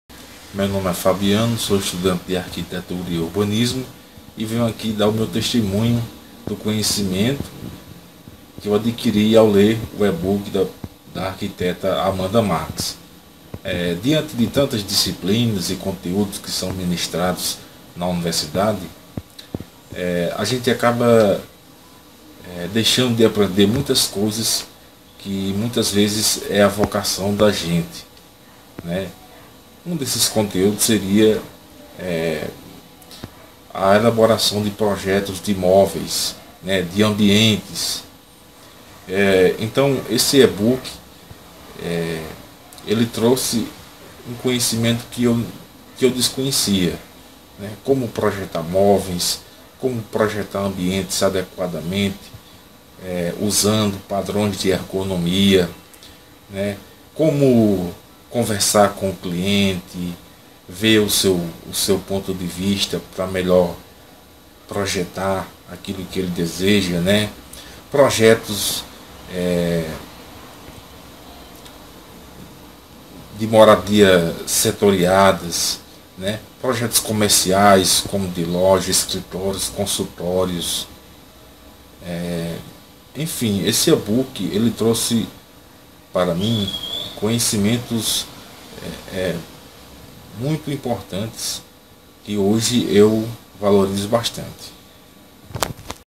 Depoimento sobre Amanda Marques - AM Cursos - Fabiano de Medeiros Dantas
Fabiano é estudante de Arquitetura e Urbanismo e descobriu sua grande vocação, que é projetar móveis e ambientes, através da leitura do Livro Digital "Projeto de Móveis na Prática".